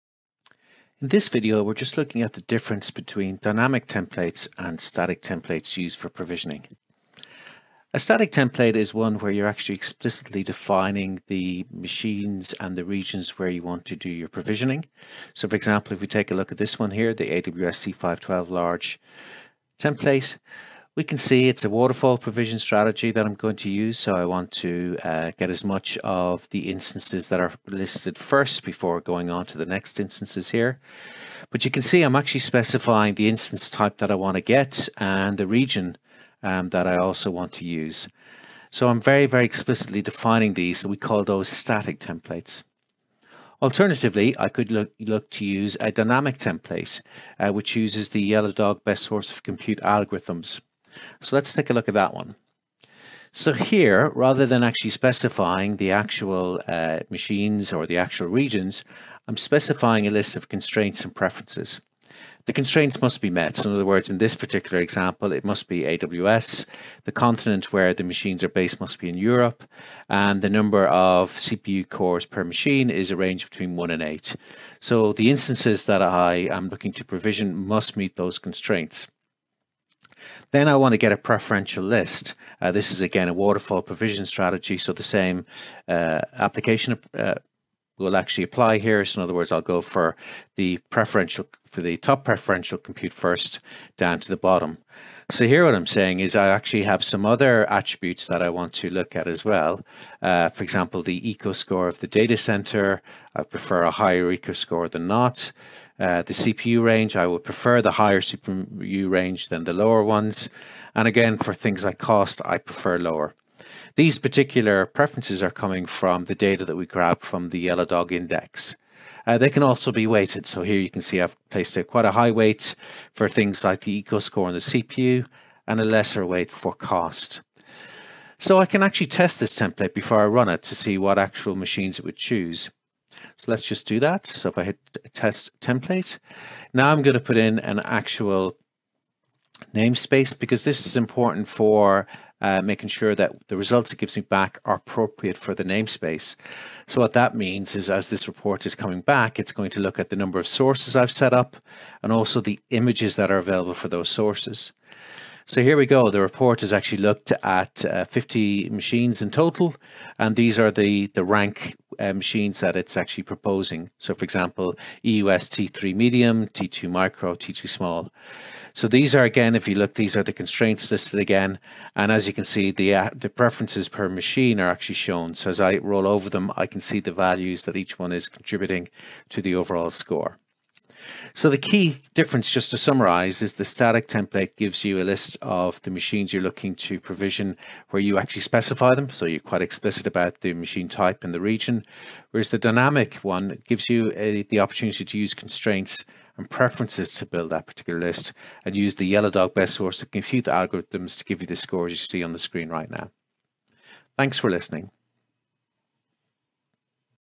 The Difference Between Static and Dynamic Provisioning Templates
In this video learn the difference between static and dynamic compute provisioning templates in the YellowDog Platform.

Are you using the best source of compute for your business?

Get in touch if you have any questions, we'd love to hear from you!